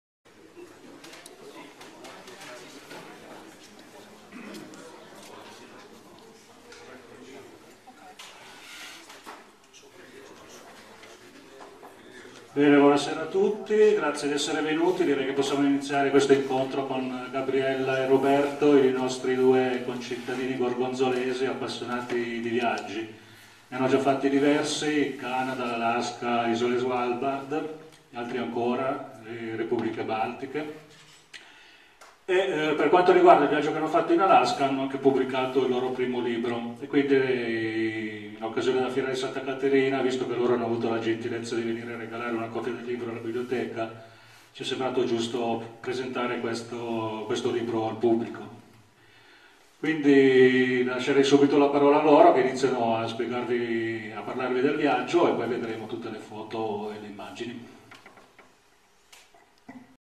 Presentazione Viaggio in Alaska: introduzione / The presentation of our book: Travelling in Alaska
Gorgonzola, 25 novembre 2011: serata di presentazione del  libro di ScriCal Viaggio in Alaska - In giro per la Kenai Peninsula, organizzata dall'Assessorato alla Cultura ed Eventi e Biblioteca di Gorgonzola. Nel video Marco Caputo, Biblioteca di Gorgonzola, introduce ScriCal (Gabriella Mazzon Venturati e Roberto Venturati) autori di Viaggio in Alaska.